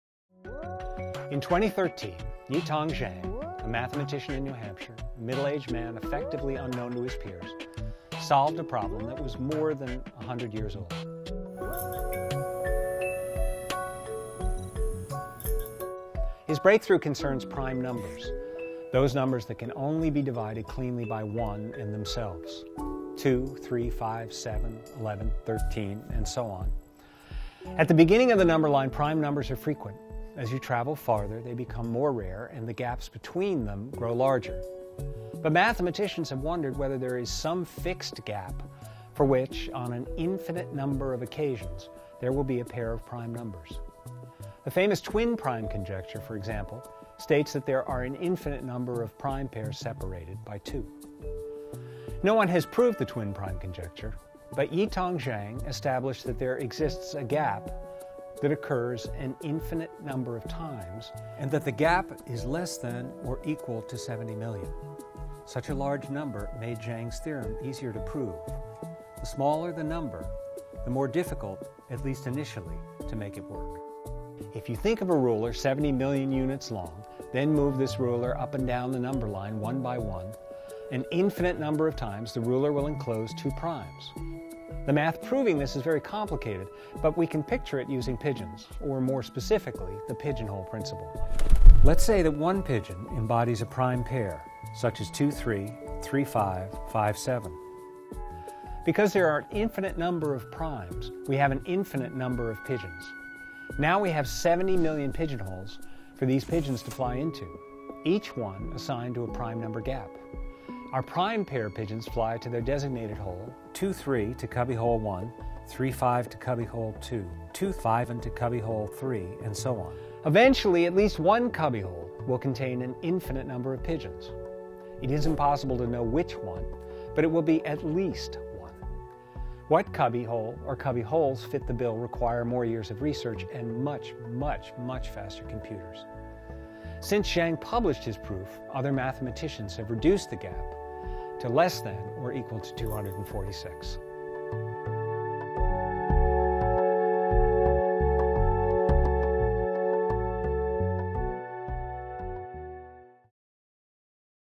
Yitang Zhang's Mathematical Discovery
Alec Wilkinson explains Yitang Zhang’s mathematical breakthrough.

Starring: Alec Wilkinson
Producer: Nate Lavey